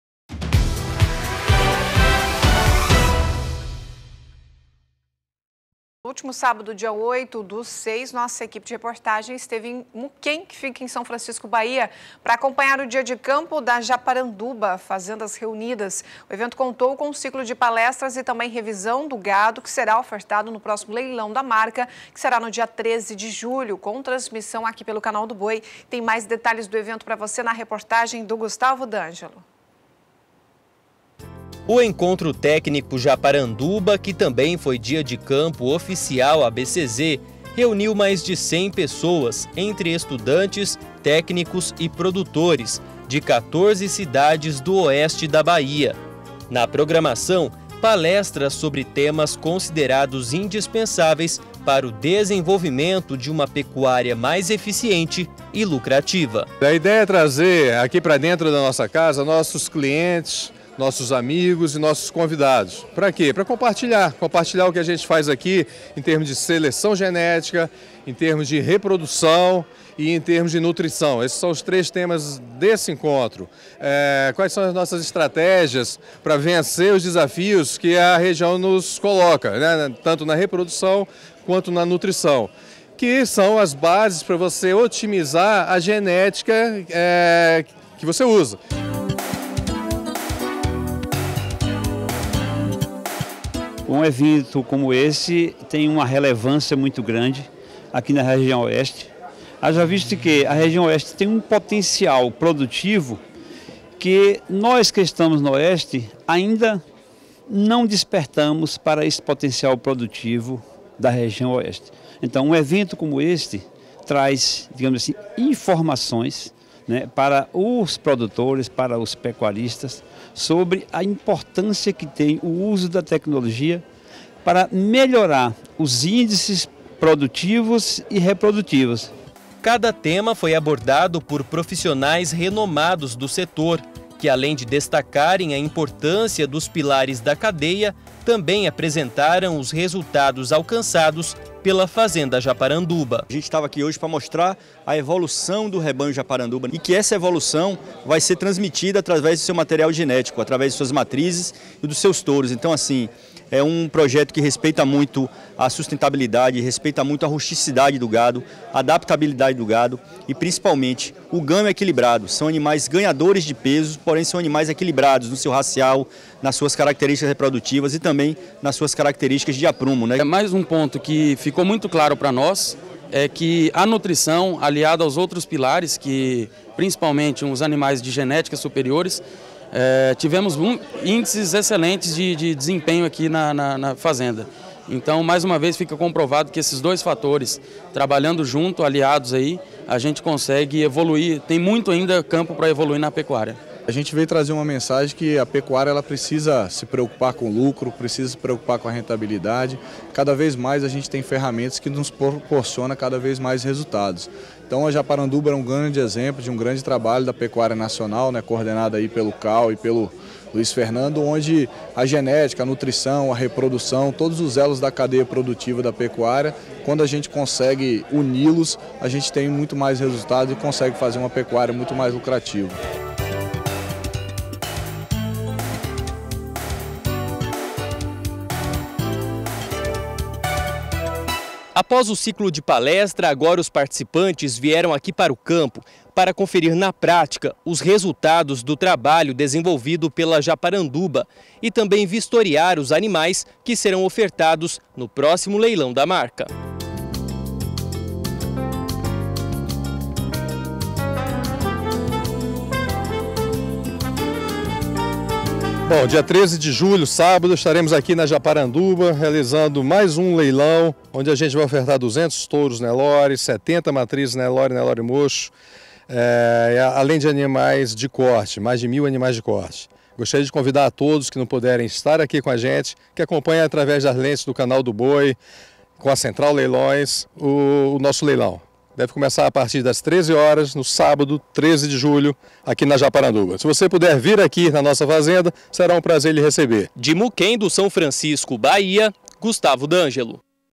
Dia de campo promovido pela fazenda Japaranduba aborda genética reprodução e nutrição
No último sábado (08/06), nossa equipe de reportagem esteve em Muquém do São Francisco-BA para acompanhar o dia de campo da Japaranduba Fazendas Reunidas. O evento contou com um ciclo de palestras e também revisão do gado que será ofertado no próximo leilão da marca, que acontece no dia 13 de julho, com transmissão aqui pelo Canal do Boi. Confira como foi o evento na reportagem do Gustavo D’Angelo.